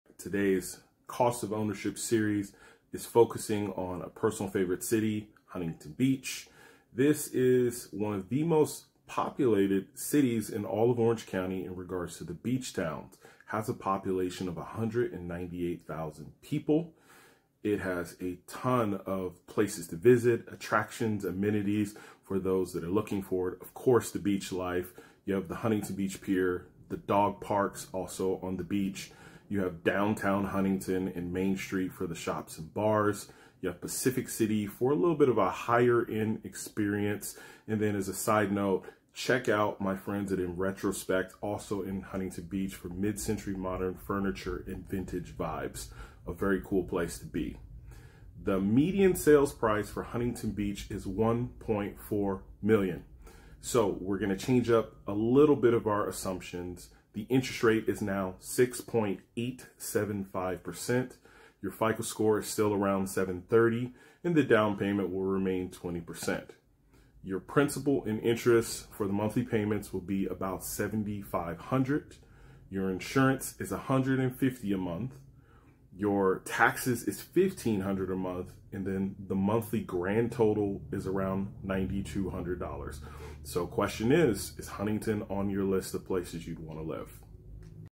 How Much Would it Cost to Buy in Huntington Beach?
How much does it really cost to buy a home in Orange County? In today's video I cover the monthly cost for Huntington Beach. This is the most populous beach city in the OC. Great schools, amazing beaches, and alluring options for night life enthusiasts. Does the price of homes in Huntington Beach seem like a great value or highway robbery?

🟩 What's my home worth?